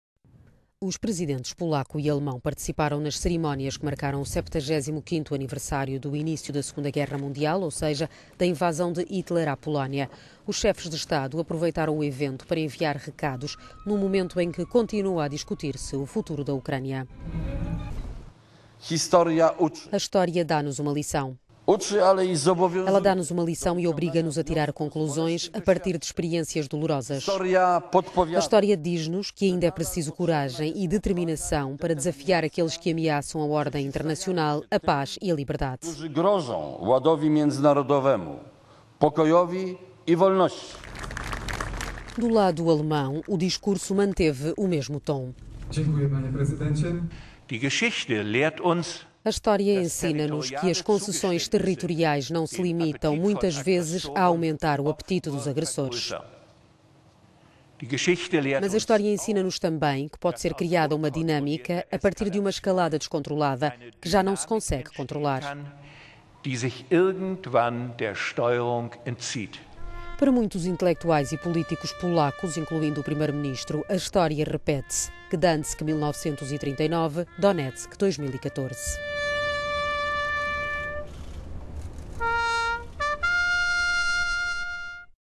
No 75º aniversário do início da II Guerra Mundial Polónia e Alemanha mandam recados sobre a Ucrânia
Os Presidentes polaco e alemão participaram nas cerimónias que marcaram o 75º aniversário do início da II Guerra Mundial. Ou seja, da invasão de Hitler à Polónia.

Os chefes de Estado aproveitaram o evento para enviar recados num momento em que continua a discutir-se o futuro da Ucrânia:

"A história dá-nos uma lição. Ela dá-nos uma lição e obriga-nos a a tirar conclusões a partir de experiências dolorosas, a história diz-nos que ainda é preciso coragem e determinação para desafiar aqueles que a…